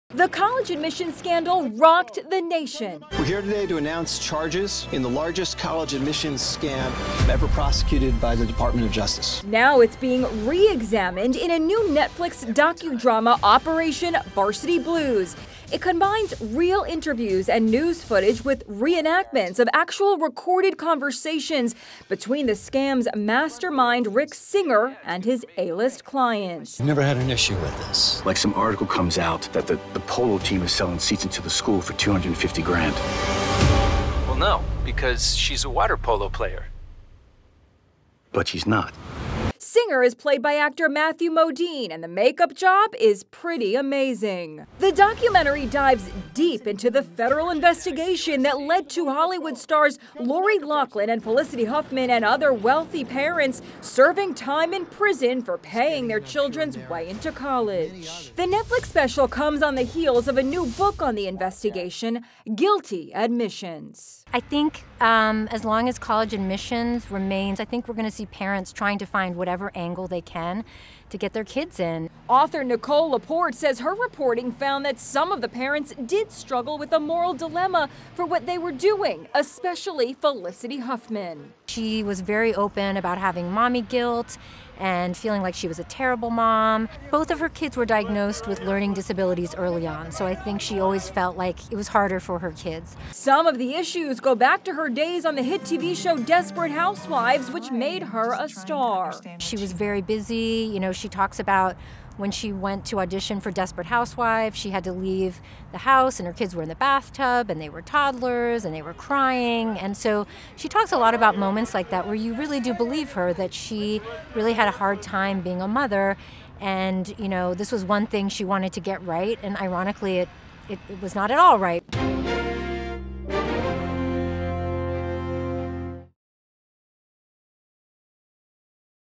Why Felicity Huffman ‘Felt Like a Terrible Mom’
It’s been nearly two years since federal agents broke the college admissions scandal that resulted in dozens of parents facing criminal charges, including actresses Lori Loughlin and Felicity Huffman. Now the case is the subject of a new Netflix film, “Operation Varsity Blues,” and a book by Nicola Laporte called “Guilty Admissions.” Laporte said her reporting found that some of the parents did struggle with a moral dilemma over what they were doing, especially Felicity Huffman.